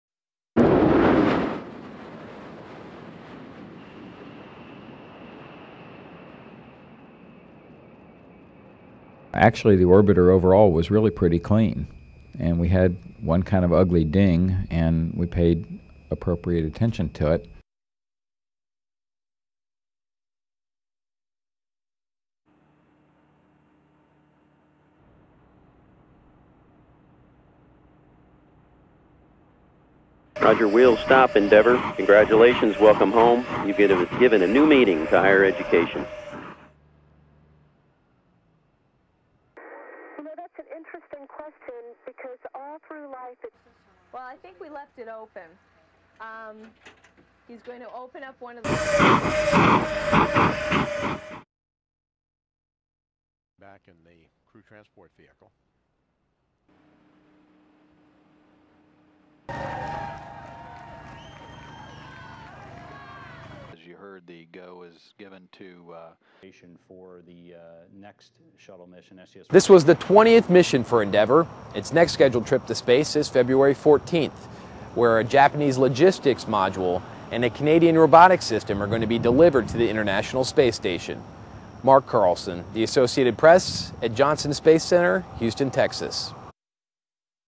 DVS Space Shuttle
(21 Aug 2007) HEADLINE: Endeavour holds up during re-entry, lands safely
CAPTION: The space shuttle Endeavour touches down safely in Florida, ending uncertainty about damage to the craft's belly. During this mission, a teacher/astronaut taught lessons in space to students on Earth. (Aug. 21)
[Notes:ANCHOR VOICE]
ENDEAVOUR LANDING 8.21.07
(12:32 PM EST video of Endeavour touching down and any Nats from the landing)
SPACE SHUTTLE ENDEAVOUR IS SAFELY ON THE GROUND AT KENNEDY SPACE CENTER…ENDING ANY UNCERTAINTY OF HOW THE CREW AND SPACE CRAFT WOULD HOLD UP DURING RE-ENTRY TO EARTH.
((SOT//SOMETHING FROM NASA PRESSER))
THE SHUTTLE WAS DAMAGED DURING LAUNCH ... WITH EITHER A PIECE OF FOAM INSULATION OR ICE FROM A BRACKET GOUGING THE CRAFT'S BELLY.
NASA WILL NOW BE ABLE TO GET A HANDS ON LOOK AT THE DAMAGE… RIGHT NOW, THE THOUGHT IS THAT NO LENGTHY POST - FLIGHT REPAIRS WILL BE NEEDED. (VO FROM TAKE-OFF, not landing)
(12:33 PM EST "CONGRATS WELCOME HOME YOU'VE GIVEN A NEW MEANING TO HIGHER EDUCATION.")
DURING THE MISSION, TEACHER/ASTRONAUT - BARBARA MORGAN TOUGHT STUDENTS FROM IDAHO, VIRGINIA AND CANADA
SHE'S KNOWN FOR BEING CHRISTA MCAULIFFE 'S BACK-UP FOR THE DOOMED CHALLENGER MISSION IN 19-86. (there is file video of the two on "NASA SHUTTLE US on EM 4505)
12:48 (NATS OF ENGINE POWER DOWN)
AFTER MORE THAN FIVE MILLION MILES OF TRAVEL THE CREW IS FINALLY BACK TO WHERE THEY STARTED.
THE UNCERTAINTY OF HURRICANE DEAN'S PATH CAUSED NASA TO LAND A DAY EARLY.
THE SEVEN MEMBER CREW LED BY COMANDER SCOTT KELLY COMPLETED A 2 WEEK LONG MISSION TO THE INTERNATIONAL SPACE STATION…WHERE ASTRONAUTS INSTALLED A GYROSCOPE AND A NEW TRUSS SEGMENT. (walk-out from day of launch)
(MARK CARLSON, STANDUP, AT JOHNSON SPACE CENTER.)
THIS IS THE 20TH MISSION FOR ENDEAVOUR AND ITS NEXT SCHEDULED TRIP TO SPACE IS FEBRUARY 14TH.
A JAPANESE LOGISTICS MODULE AND A CANADIAN ROBOTICS SYSTEM ARE GOING TO BE DELIVERED TO THE INTERNATIONAL SPACE STATION.
MARK CARLSON, THE ASSOCIATED PRESS, HOUSTON, TEXAS.